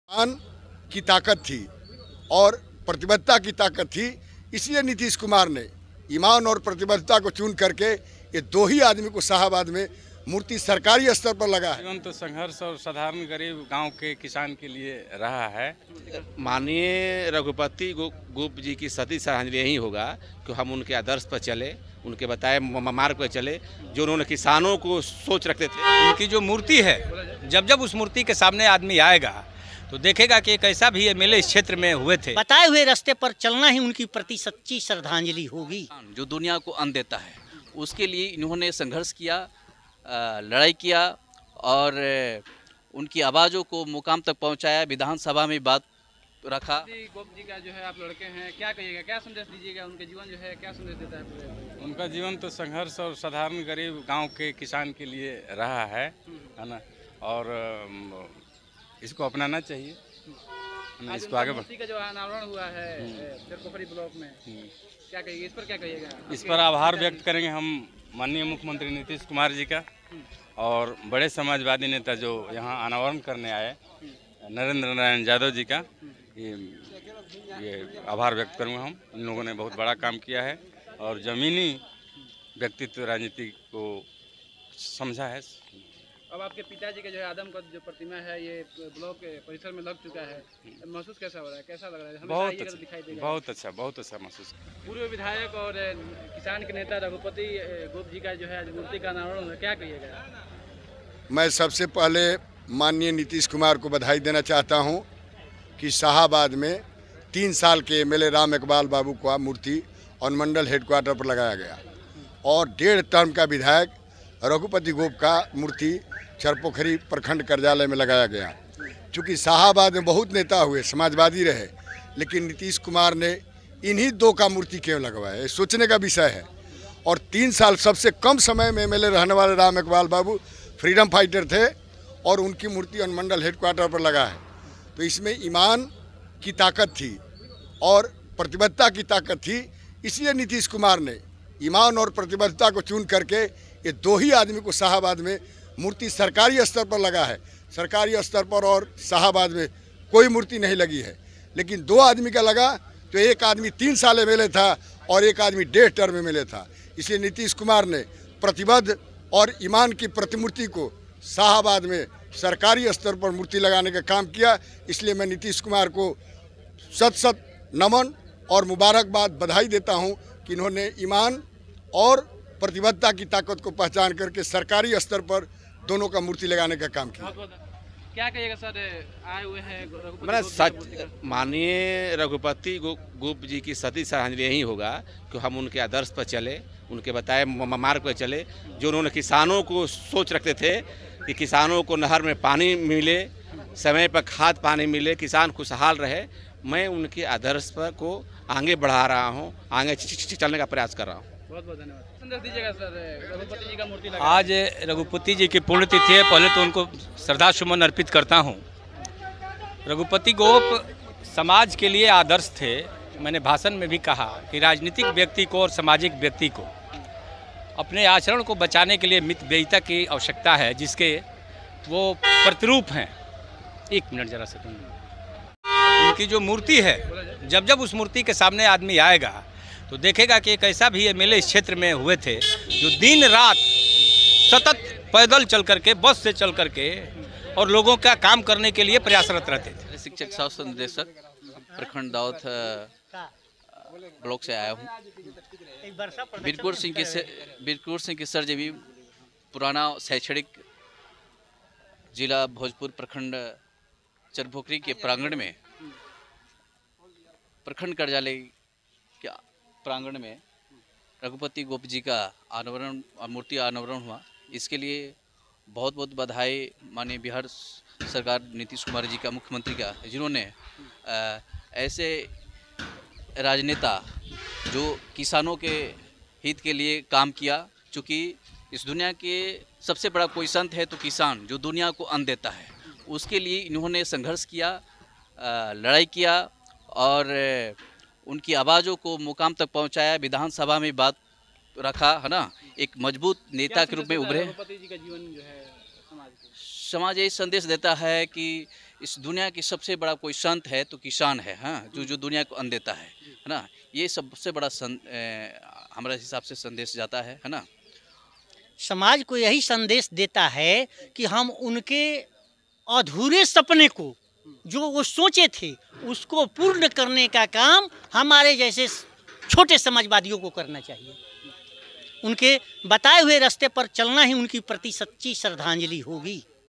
किसान नेता रघुपति गोप जी का खूब सराहना किए लोग,मूर्ति का अनावरण से खुश | ara | farmer | bhojpur
किसान नेता रघुपति गोप जी का खूब सराहना किए लोग,मूर्ति का अनावरण से खुश